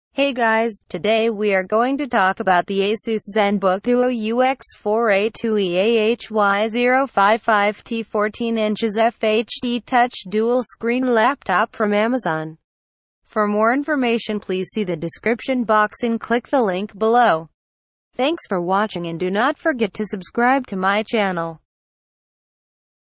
Asus Zenbook Duo UX482EA-HY055T 14" FHD TOUCH Dual Screen Laptop
Hay guys,
Today we are going to talk about the Asus Zenbook Duo UX482EA-HY055T 14" FHD TOUCH Dual Screen Laptop from Amazon.